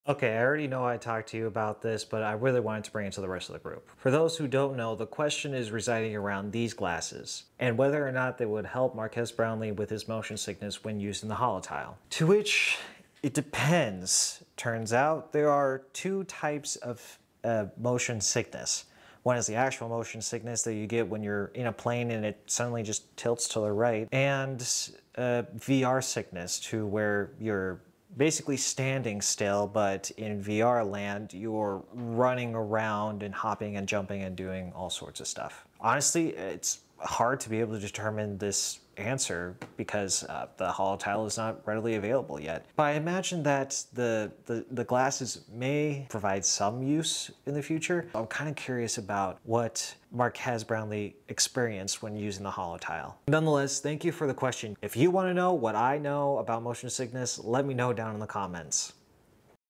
Could Glasses Cure VR motion sickness?
Could glasses cure VR motion sickness? Join us as we explore the intriguing question of whether glasses could potentially help alleviate motion sickness in virtual reality.